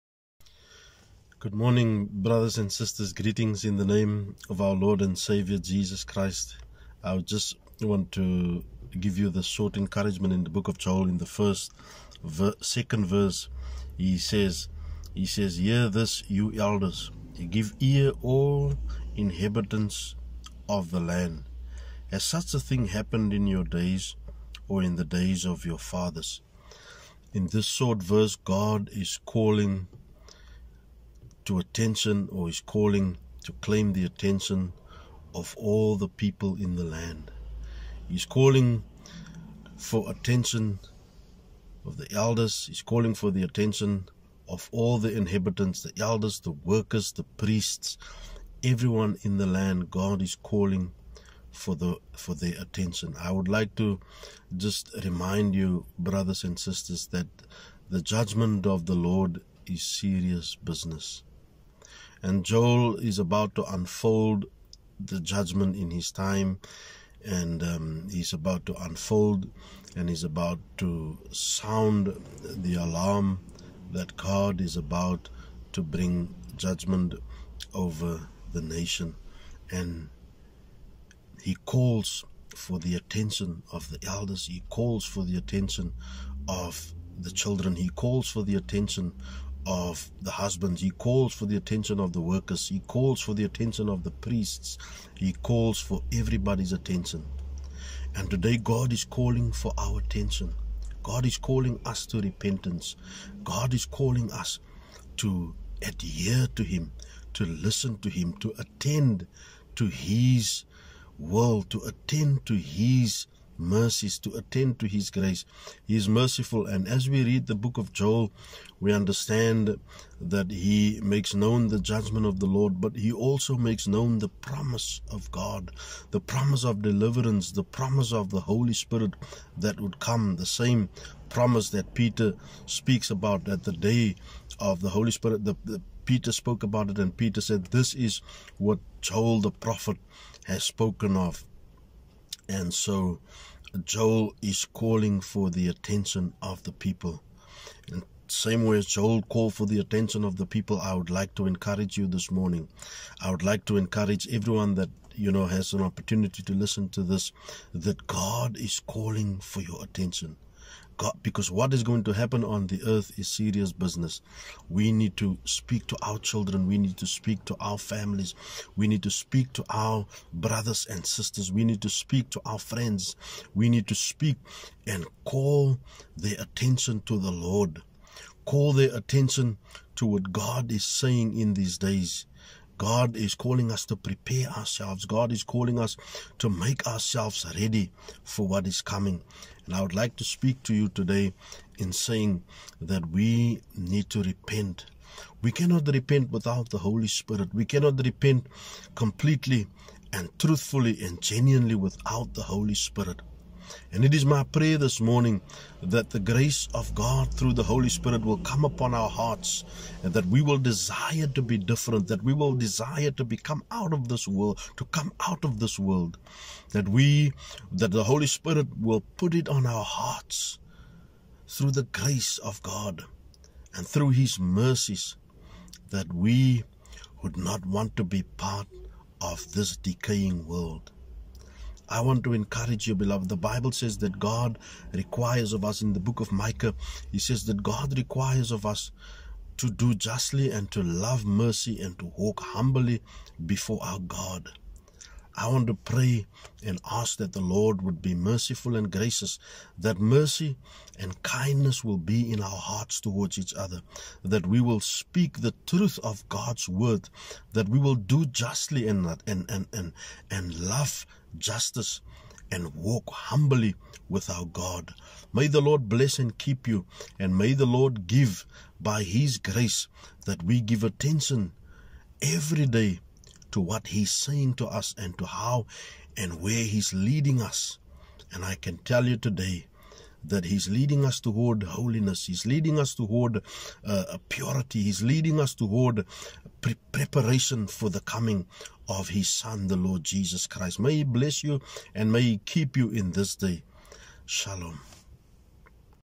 A Word of Encouragement: ‘God’s Judgment is Serious Business’ Pastor Mark Lewis - Mirrored
I just felt led by the Holy Spirit to share this massage with you brethren in Christ.

“For as many as are led by the Spirit of God, they are the sons of God. For ye have not received the spirit of bondage again to fear; but ye have received the Spirit of adoption, whereby we cry, Abba, Father. The Spirit itself beareth witness with our spirit, that we are the children of God: And if children, then heirs; heirs of God, and joint-heirs with Christ; if so be that we suffer with him, that we may be also glorified together.” Romans 8:14-17 (KJV) - “Teach me to do thy will; for thou art my God: thy spirit is good; lead me into the land of uprightness.” Psalms 143:10 (KJV).